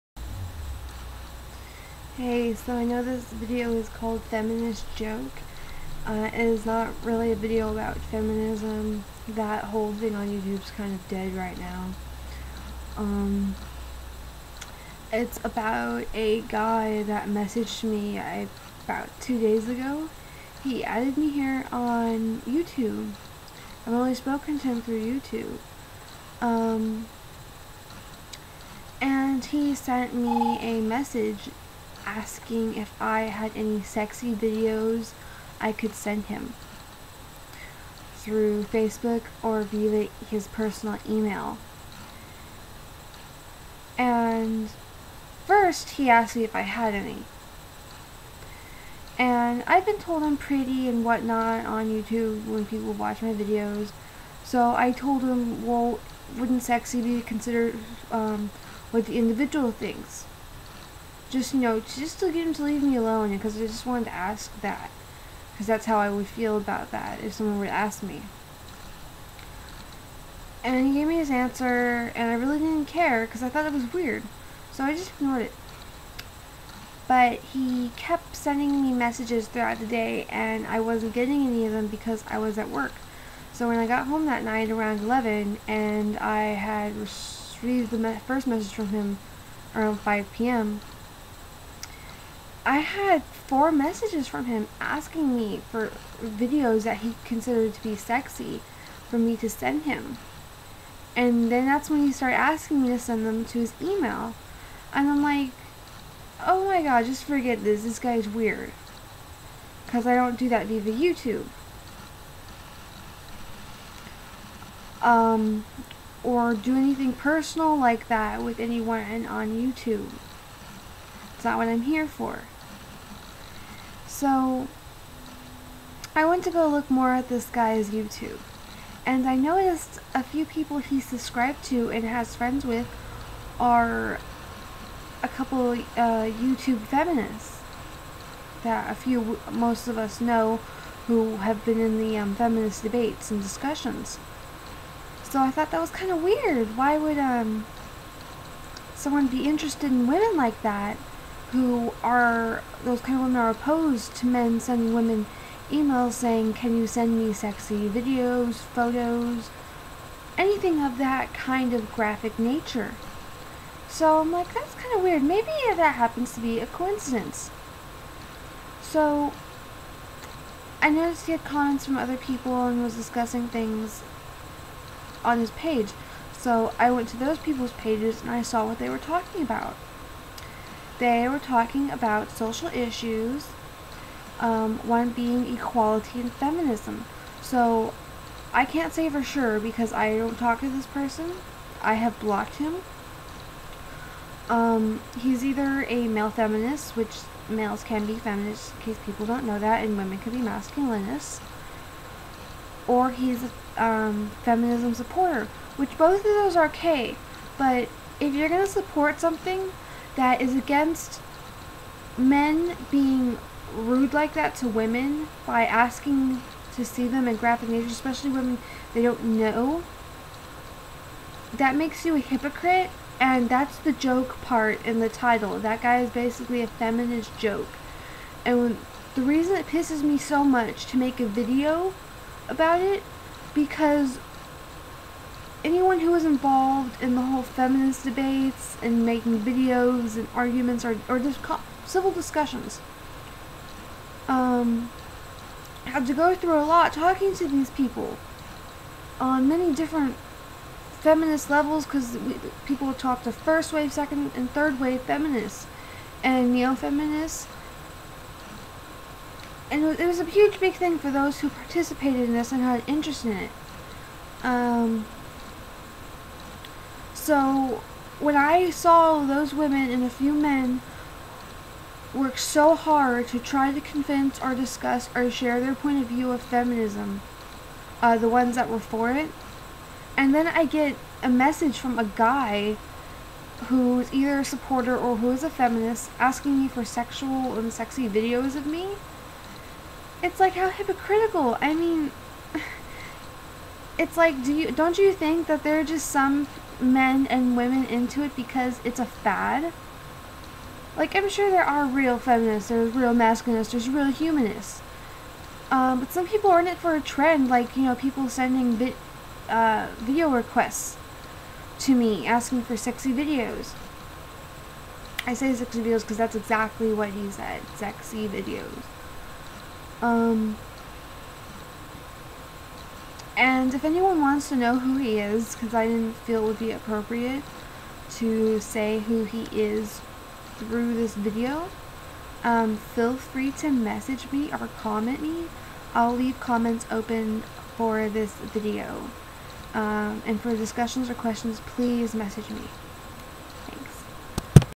Feminist Joke.
so i get this dude asking me to send him "sexy" videos via youtube, facebook, and his personal email. yea, like that was gunna happen. lol